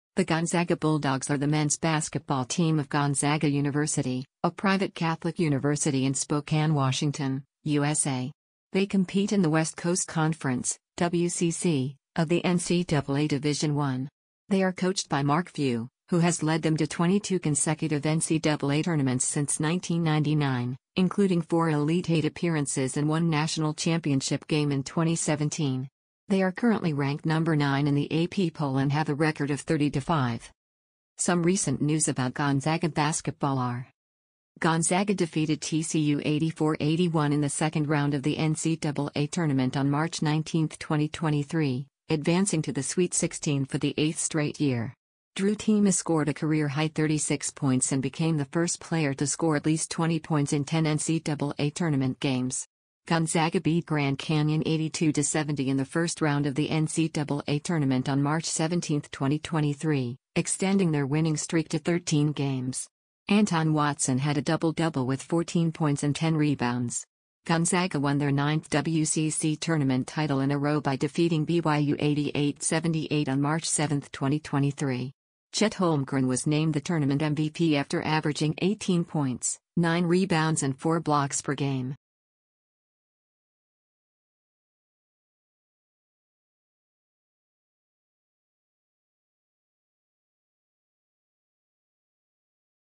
The Gonzaga Bulldogs are the men’s basketball team of Gonzaga University
The Gonzaga Bulldogs are the men’s basketball team of Gonzaga University, a private Catholic university in Spokane, Washington, USA. They compete in the West Coast Conference (WCC) of the NCAA Division I. They are coached by Mark Few, who has led them to 22 consecutive NCAA tournaments since 1999, including four Elite Eight appearances and one national championship game in 2017. They are currently ranked No. 9 in the AP poll and have a record of 30-5.

Some recent news about Gonzaga basketball are:

Gonzaga defeated TCU 84-81 in the second round of the NCAA tournament on March 19, 2023, advancing to the Sweet 16 for the eighth straight year. Drew Timme scored a career-high 36 points and became the first player to score at least 20 points in 10 NCAA tournament games1.
Gonzaga beat Grand Canyon 82-70 in the first round of the NCAA tournament on March 17, 2023, extending their winning streak to 13games. Anton Watson had a double-double with 14 points and 10 rebounds1.
Gonzaga won their ninth WCC tournament title in a row by defeating BYU 88-78 on March 7, 2023. Chet Holmgren was named the tournament MVP after averaging 18 points, nine rebounds and four blocks per game.